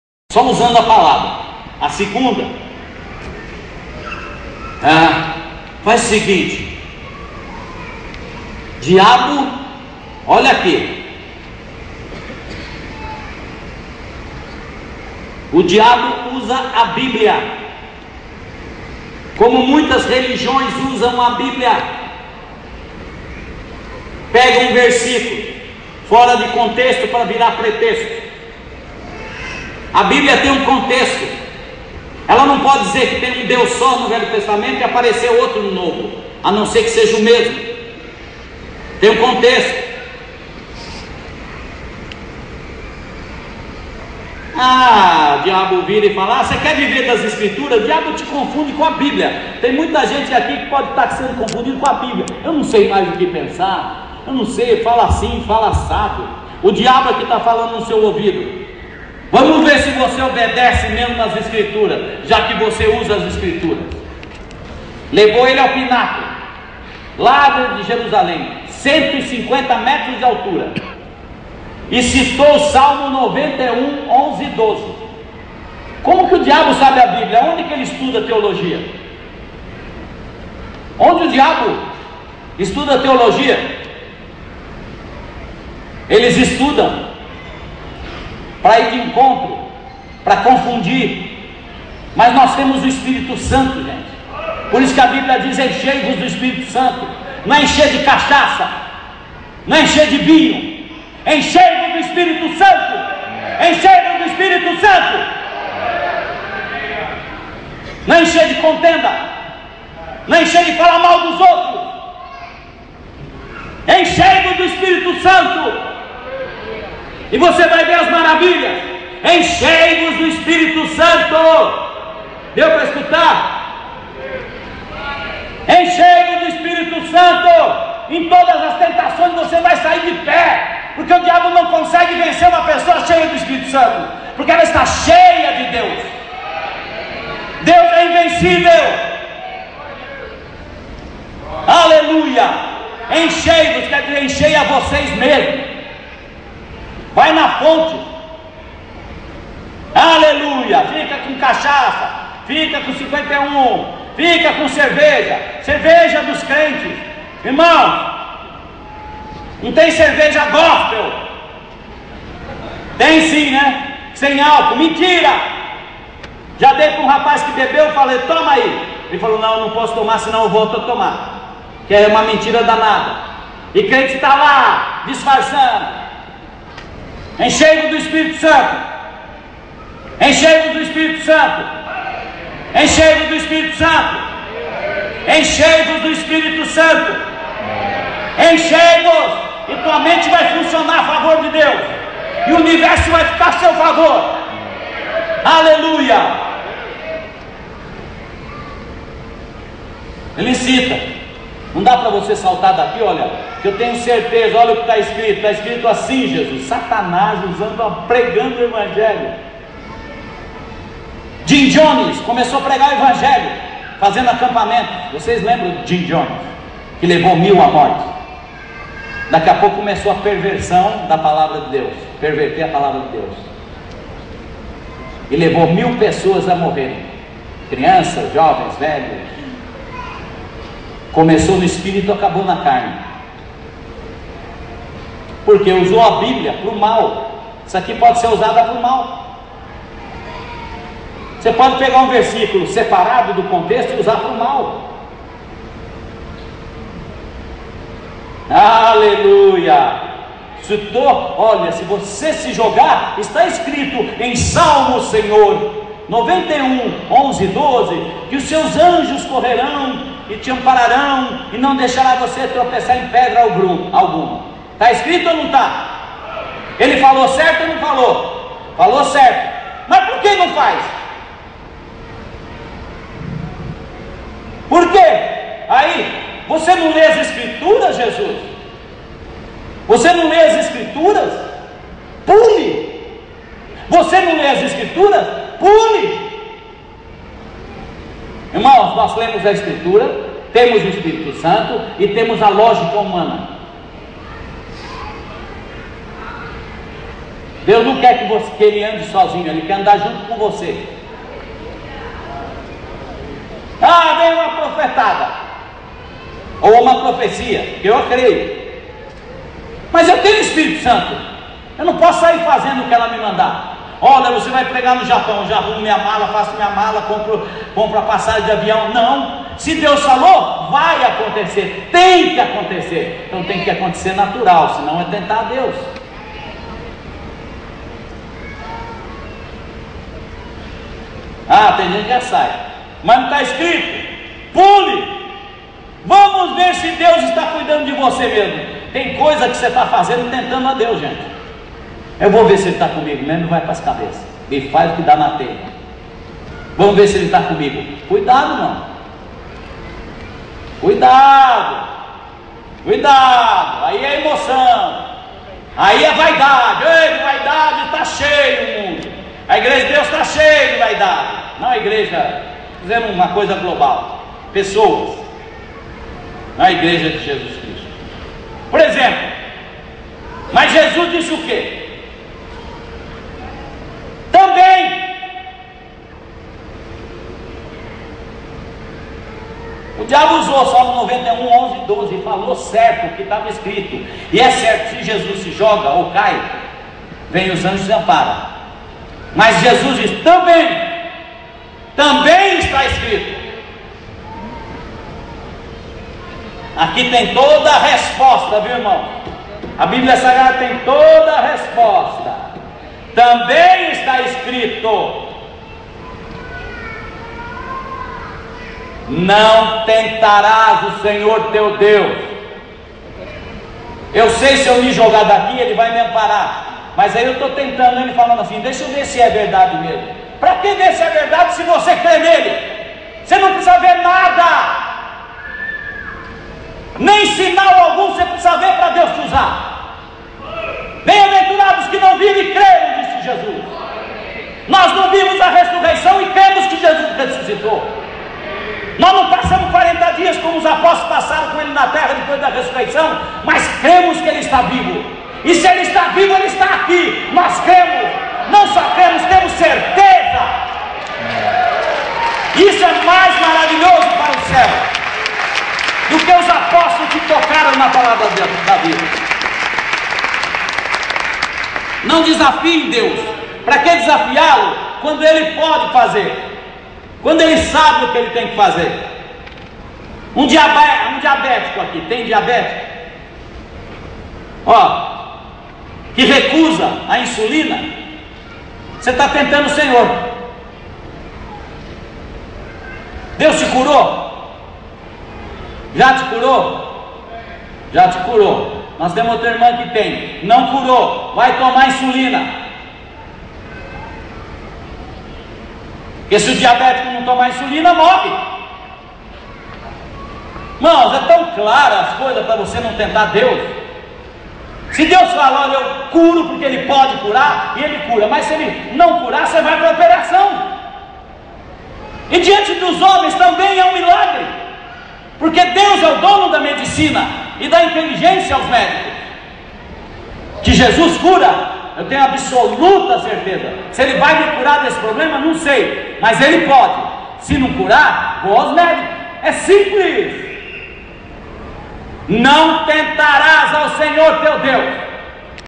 VOZ DA VERDADE-Calma diante da tentação Parte 4
Fantastica pregação ministrada pelo Pr Carlos Moysés vele a pena ver todos os videos